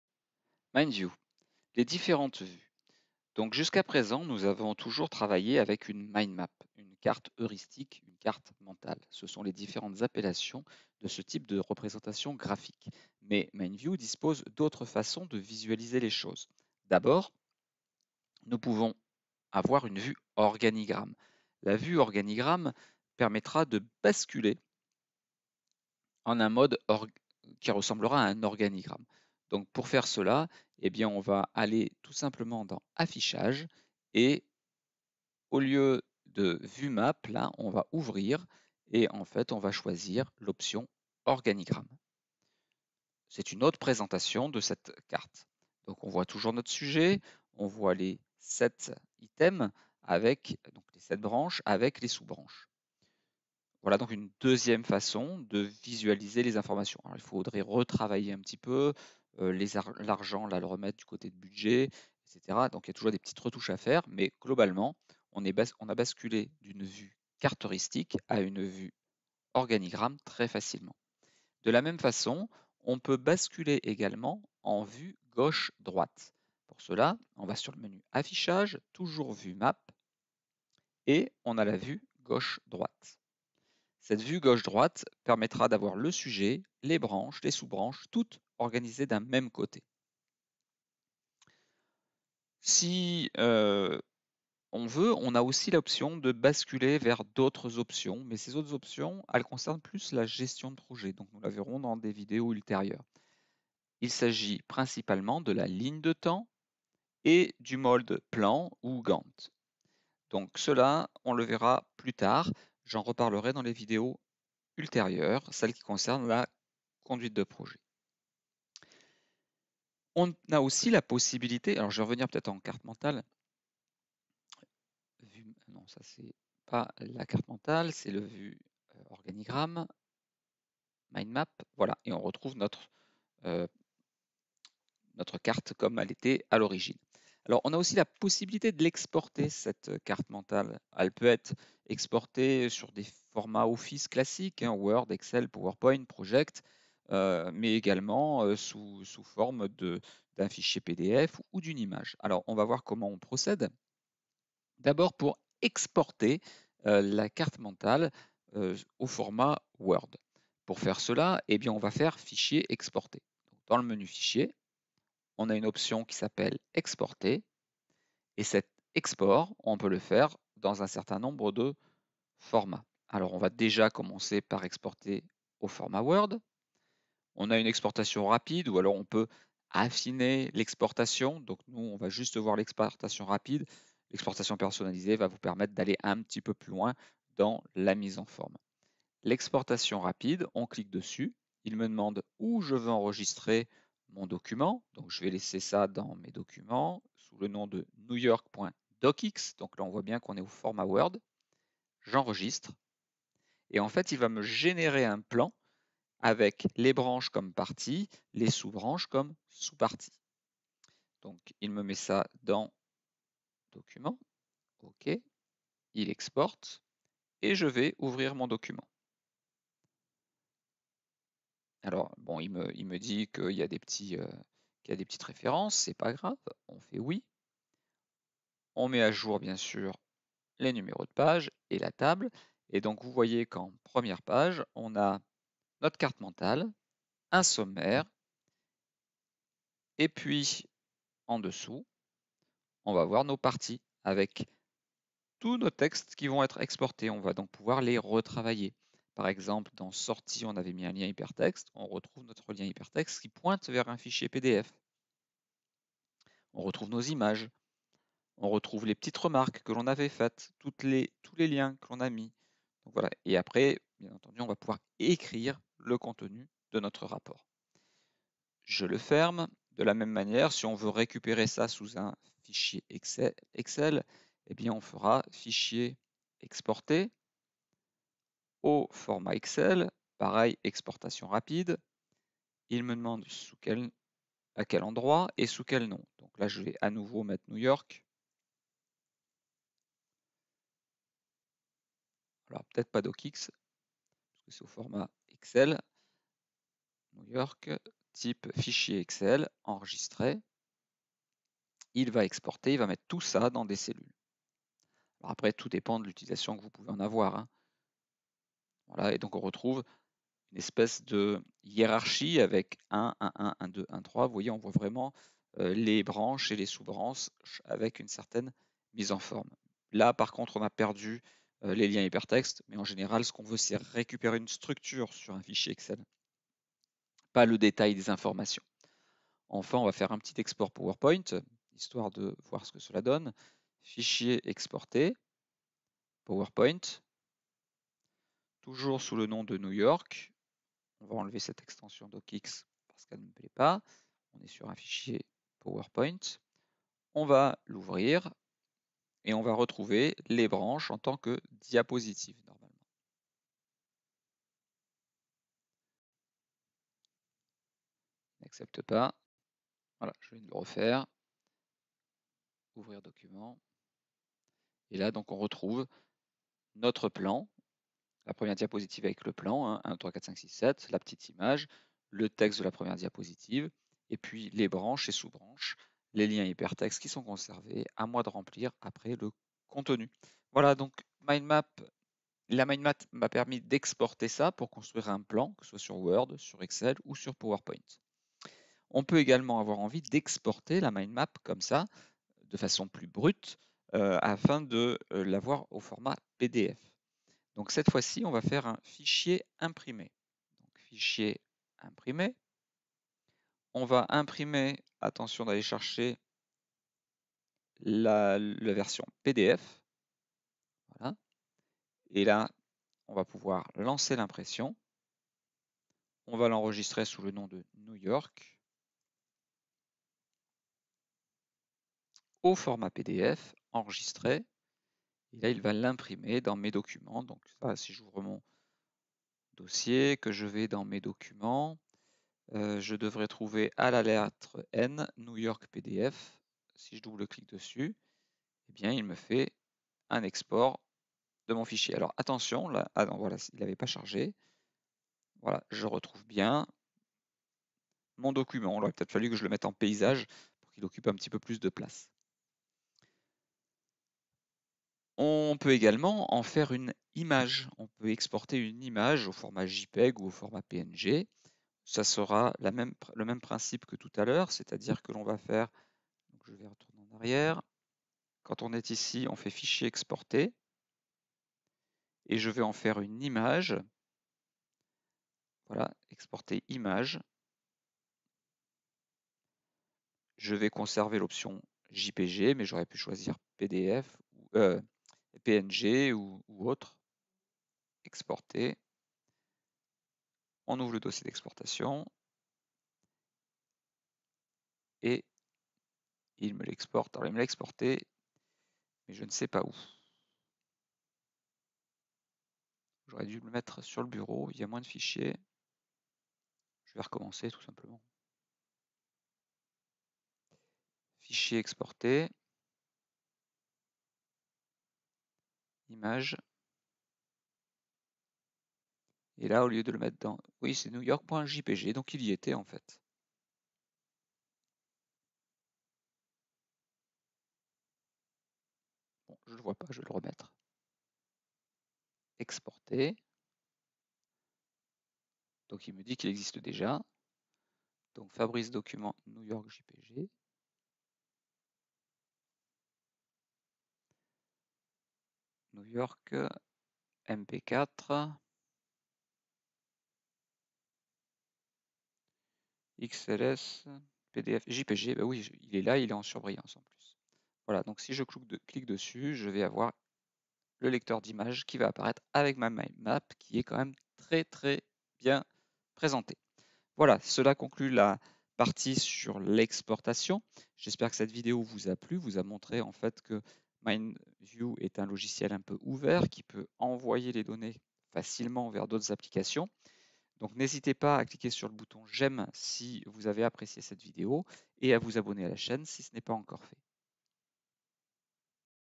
MindView 011 - Les vues et les exportations
La carte mentale peut s'afficher de différentes façon (mode organigramme et mode gauche/droite) pour améliorer la lisibilité de celle-ci.
Nous pouvons également exporter sous divers format la carte mentale (Word, Excel, Powerpoint, pdf, image,...)